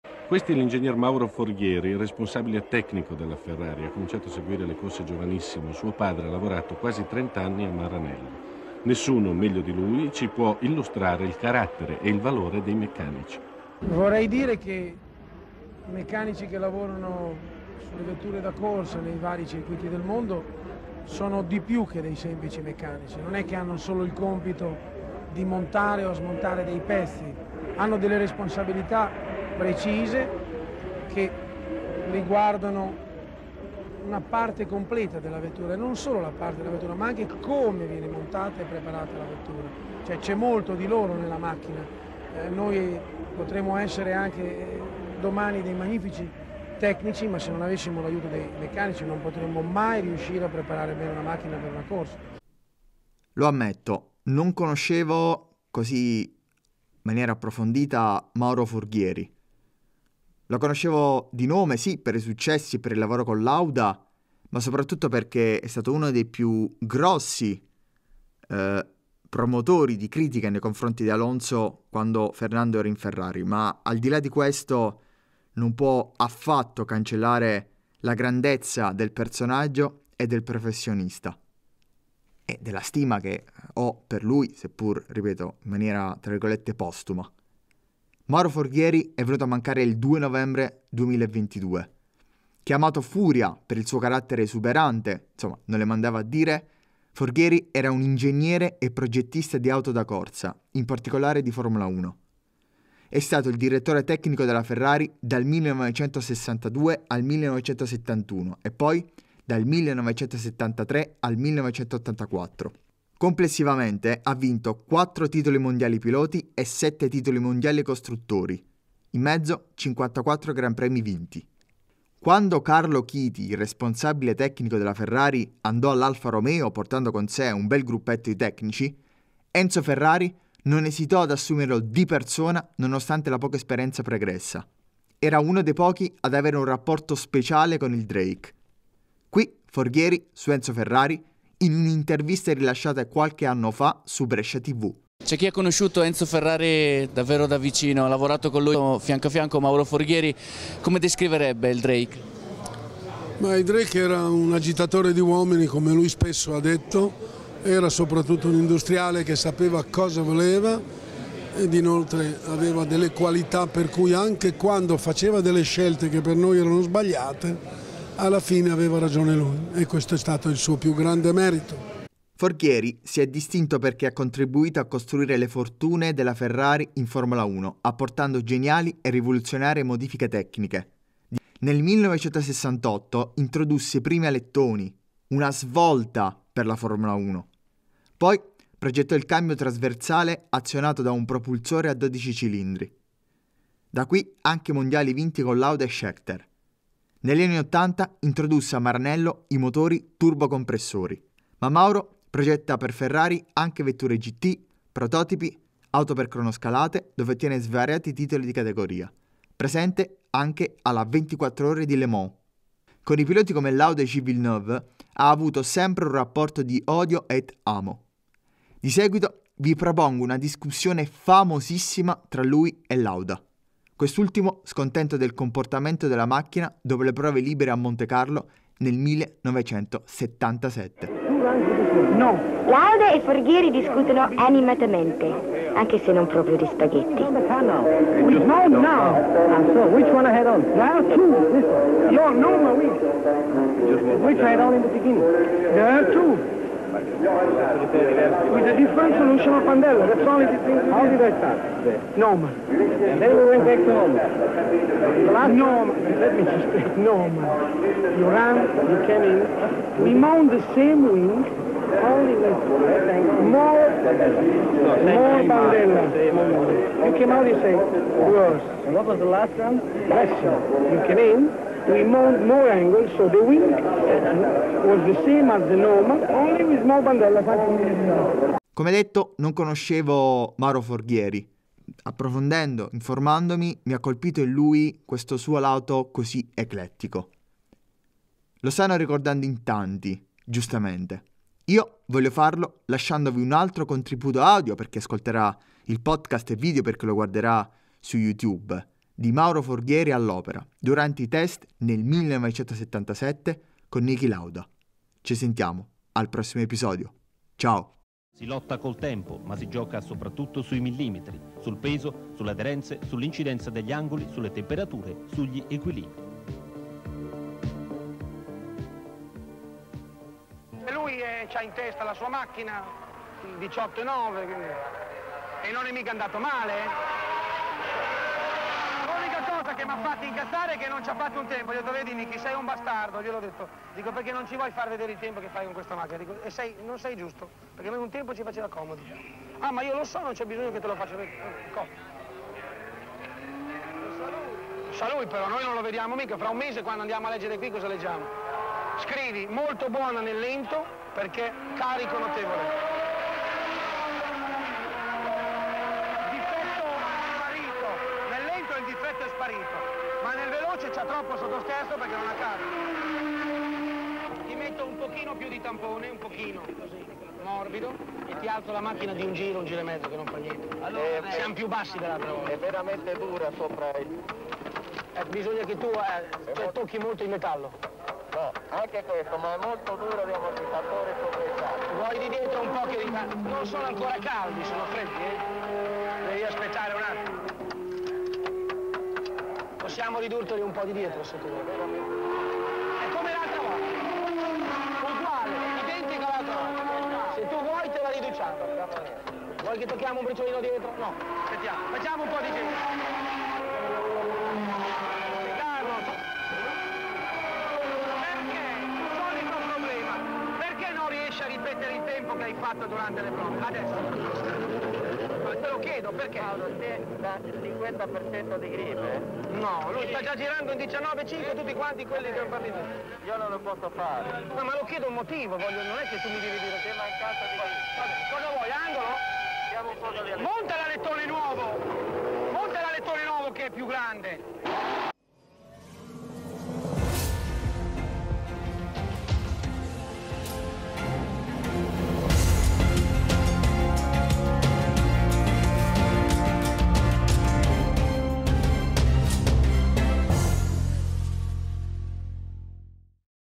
Mauro Forghieri, il genio di Maranello
Mauro Forghieri è venuto a mancare il 2 novembre 2022. Il genio di Maranello ha contribuito a costruire le fortune della Ferrari in F1 apportando geniali e rivoluzionarie modifiche tecniche. Personaggio fumanti aveva confidenza con Enzo Ferrari che lo assunse di persona. Il mio tributo ad un grande personaggio della Formula 1.

Formula Talk è il podcast che parla di Formula 1 fatto da appassionati per appassionati.